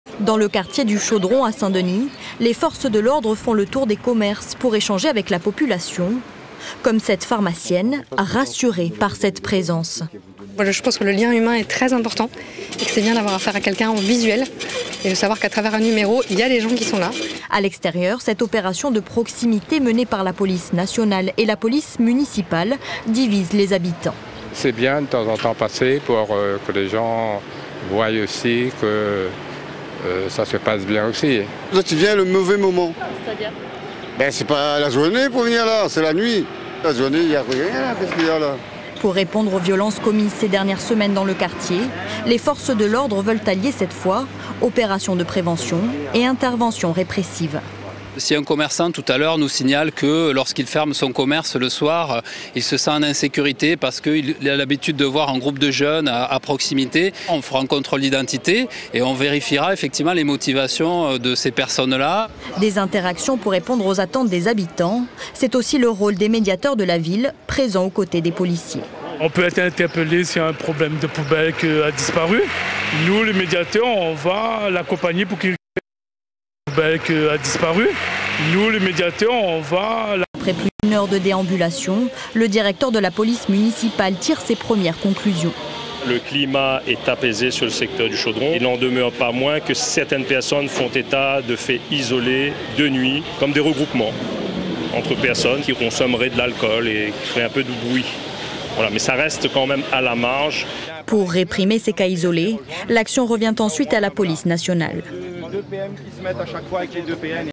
Une opération de lutte contre la délinquance a eu lieu au Chaudron.
Une opération de lutte contre la délinquance a eu lieu au Chaudron. Ces derniers jours, des violences ont éclaté dans des quartiers de Saint-Denis. Reportage